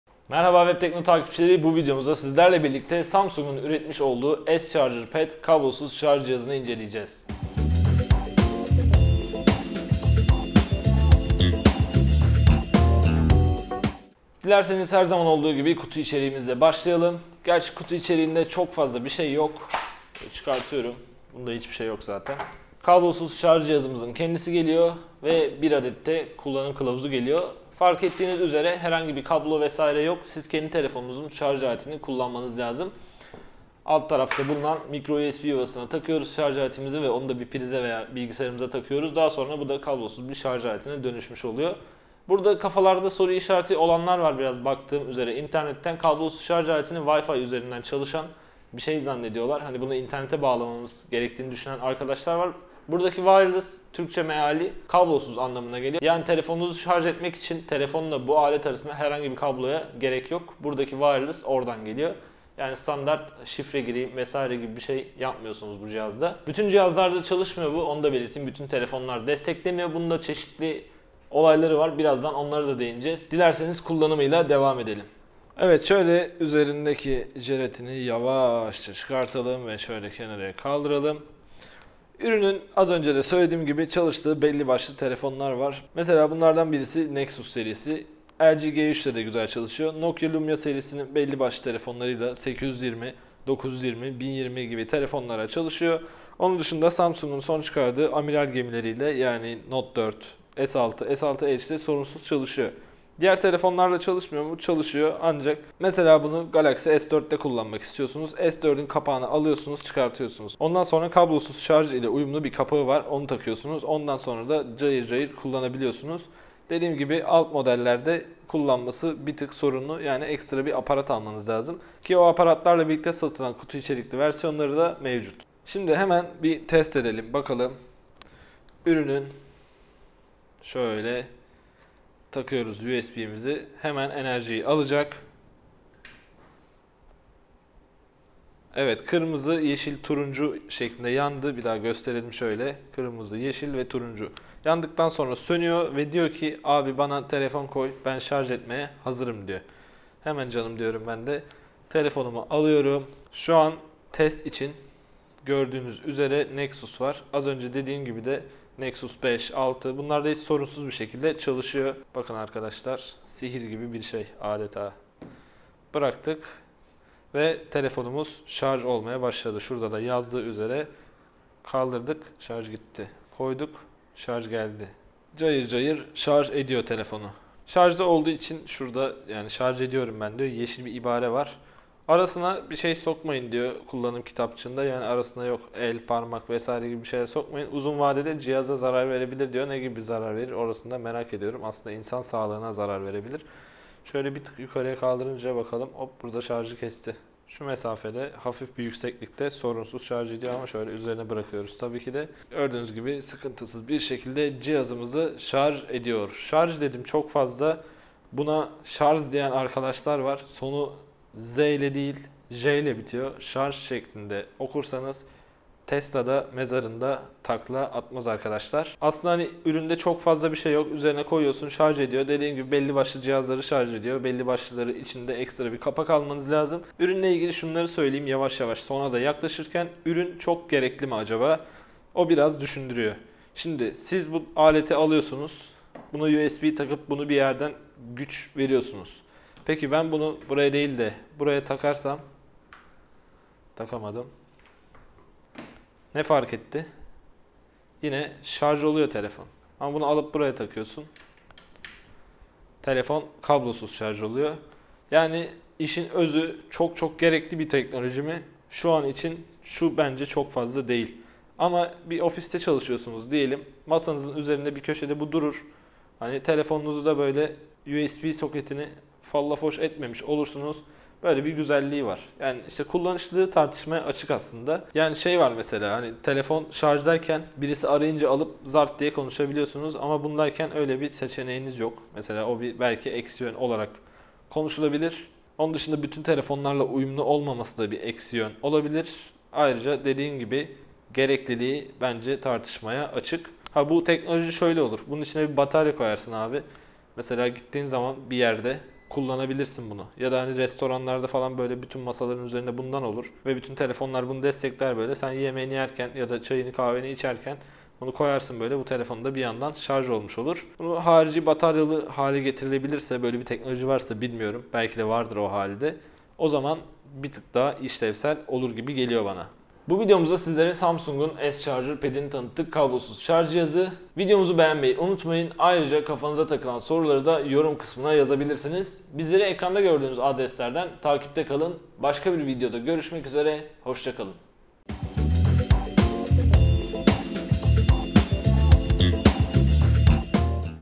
Kablosuz Şarj Cihazı İncelemesi - Samsung S Charger Pad
Bu videomuzda Samsung'un ürettiği, mobil cihazlarımızı kablosuz bir şekilde şarj eden S Charger Pad'i inceliyoruz.